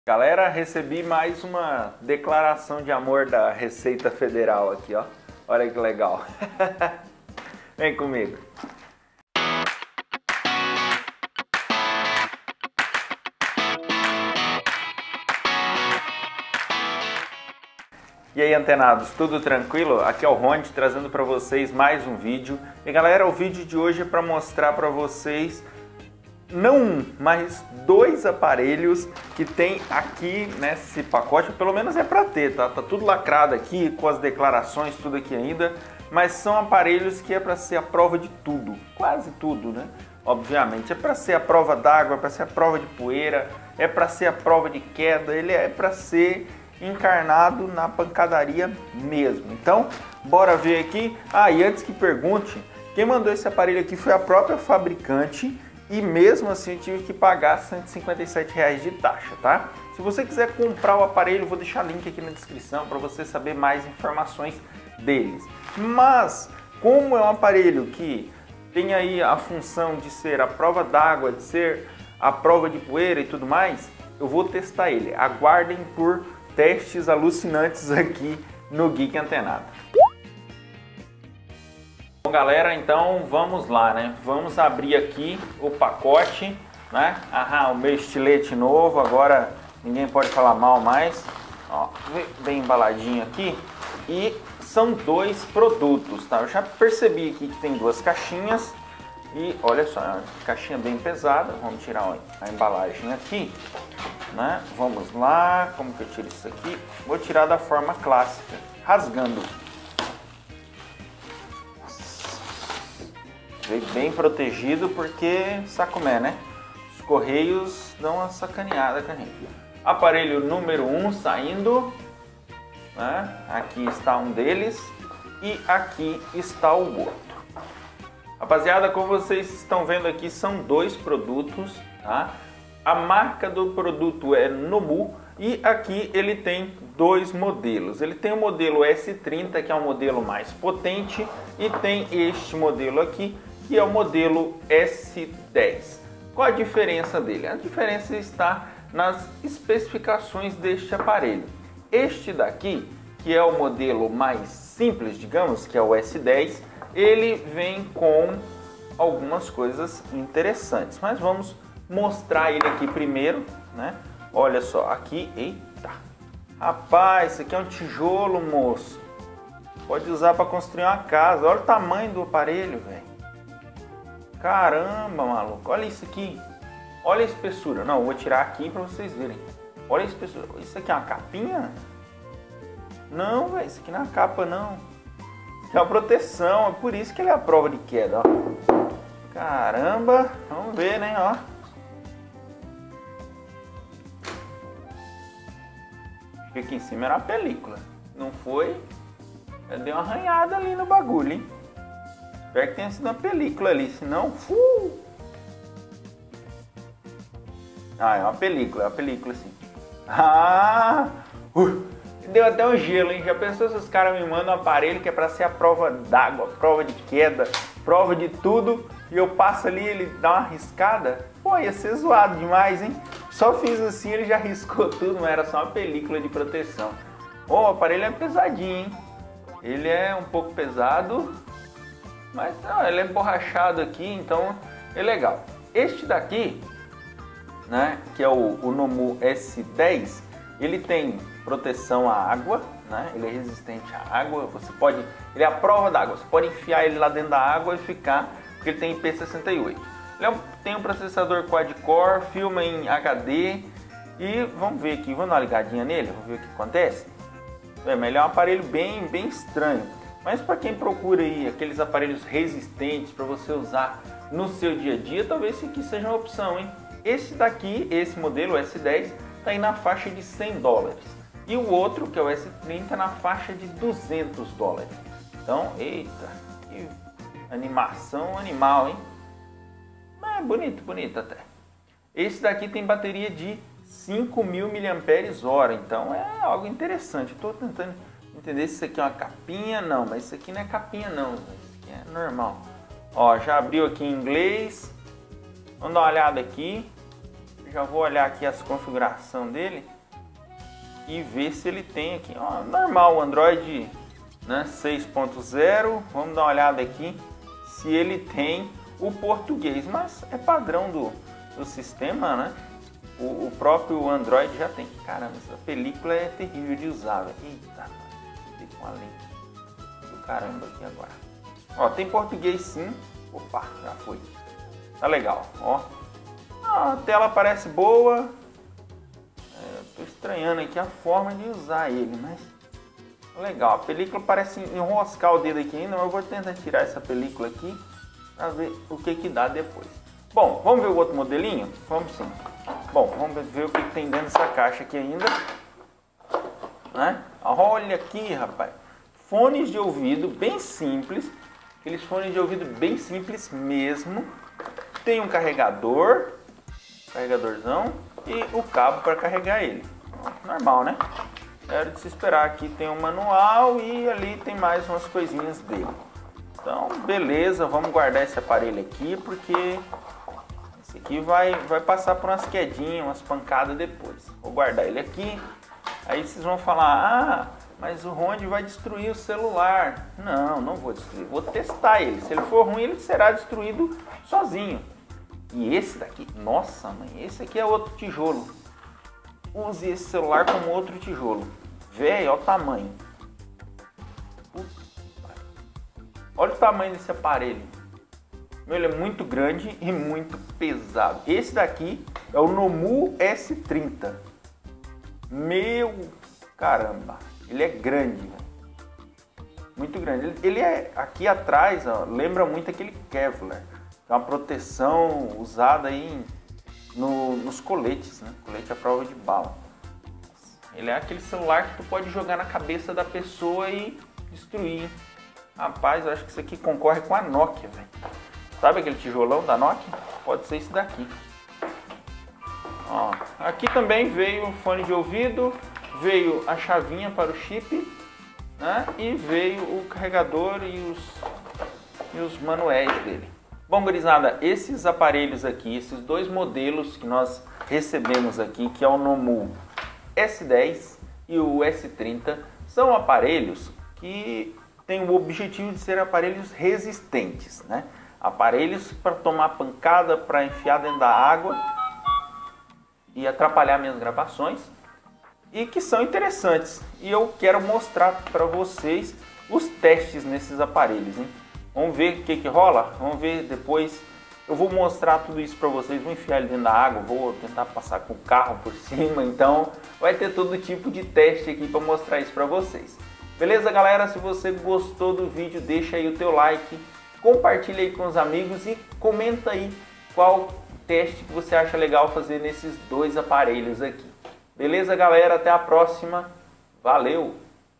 Smartphone SUPER RESISTENTE || Nomu S10 e Nomu S30 (Unboxing)
geekantenado Este vídeo é para mostrar 2 aparelhos que são muito resistentes para o dia a dia. Estes aparelhos são a prova d`água, a prova de poeira, resistente a quedas e tela muito forte. Confira o vídeo do Unboxing e saiba mais dos aparelhos.

★Conheça o CidadeTech, o canal que une os melhores YouTubers de tecnologia do Brasil!